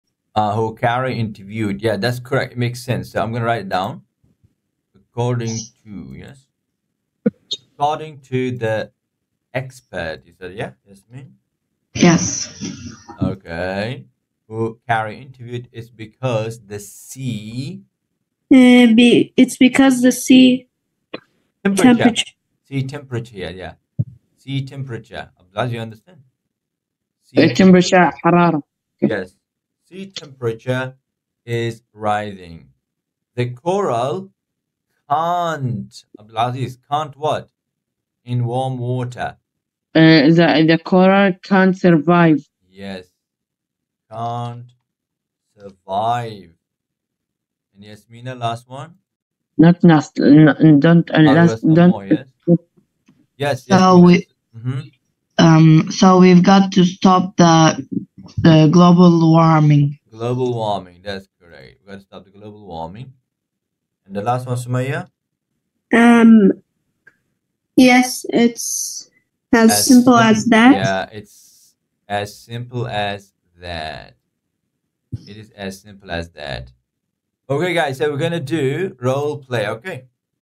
Online class, English World 8, Sample
A sample from my online class for young learners in Level 8, English World